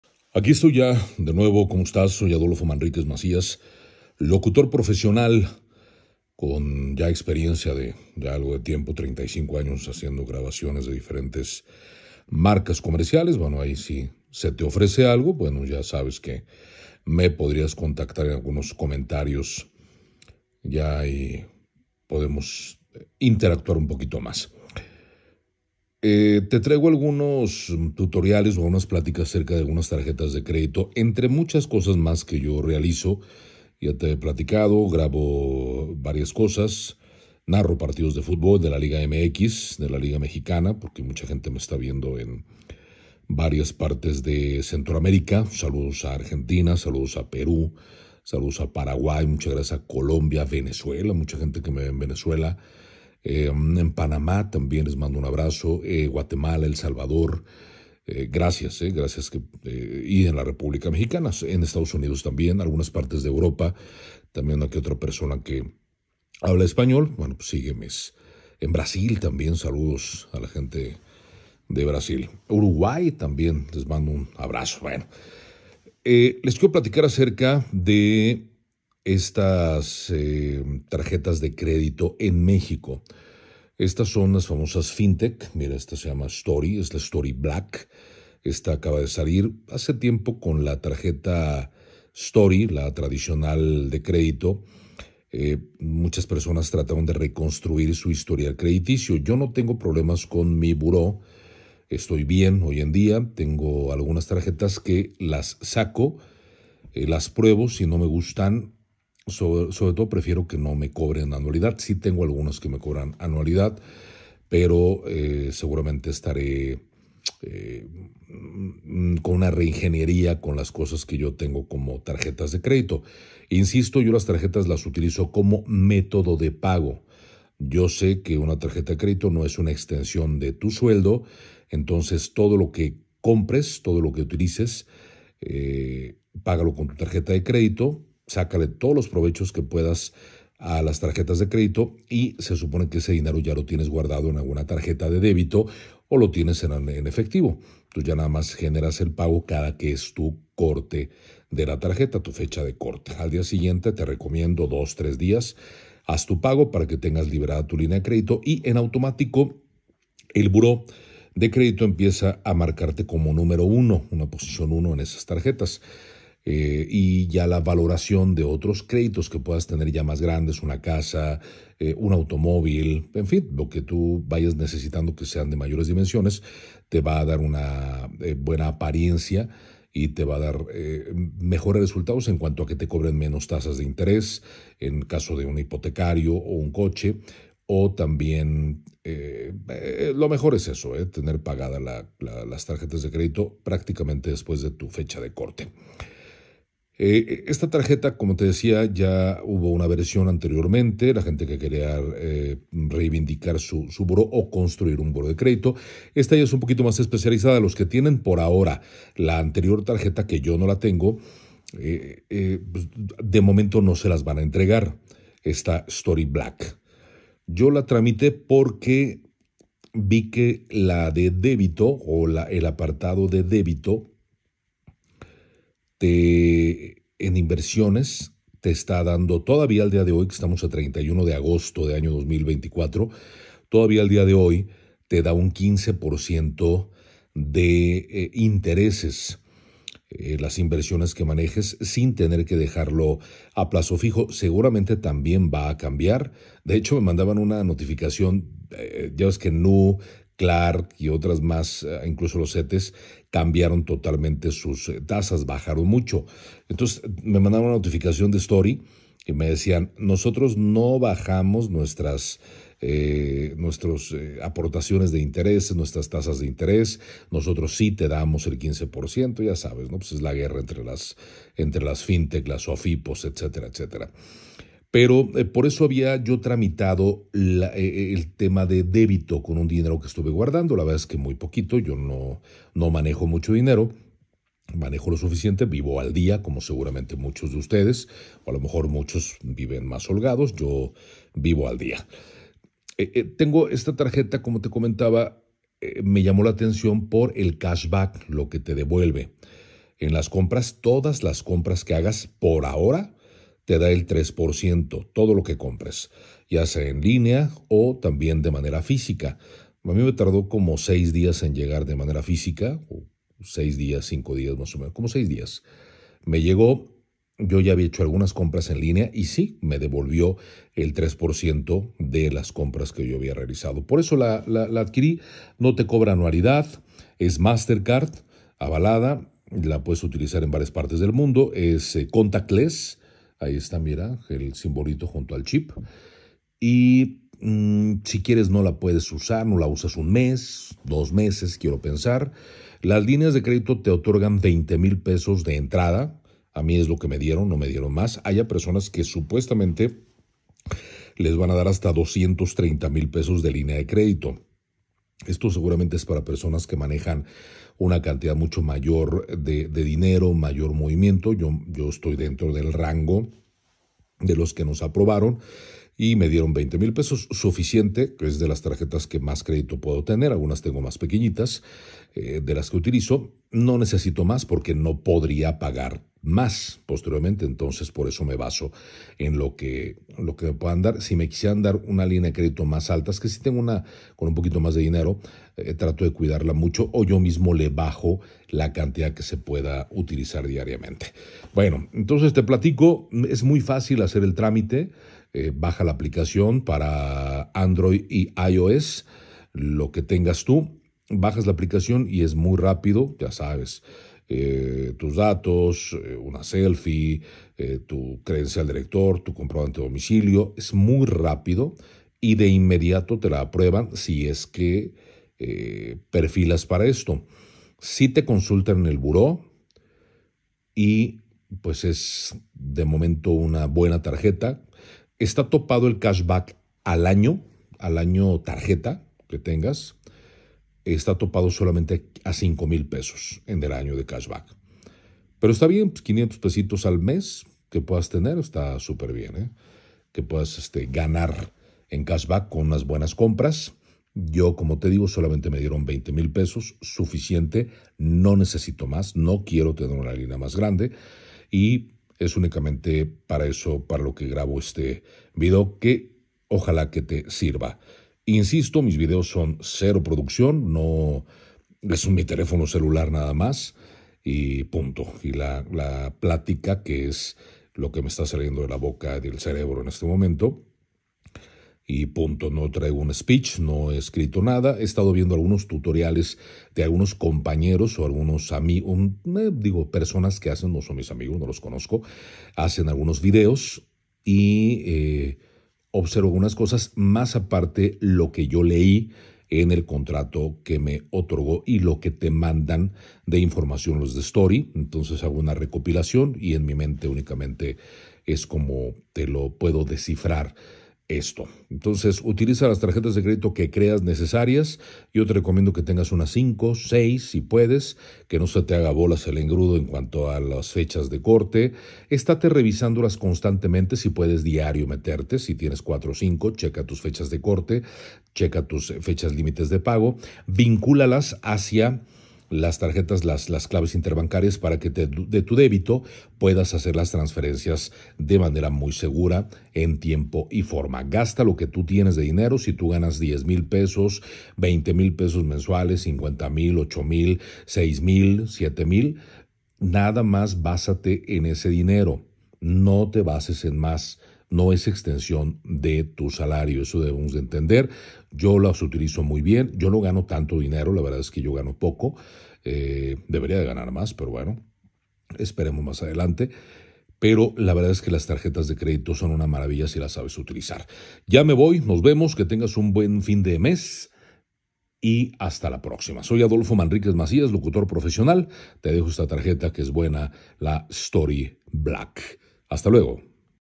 Tarjeta de crédito STORI BLACK muy esperada y te regresa dinero en todas tus compras! Buena .
Hasta el día de hoy 3 de septiembre 2024 es una tarjeta buena que te da una recompensa ya considerable del 3% en sodas tus compras ! Si te la aprueban inicia desde una línea de crédito de $20,000 veinte mil pesos , ya es para tomarse en cuenta !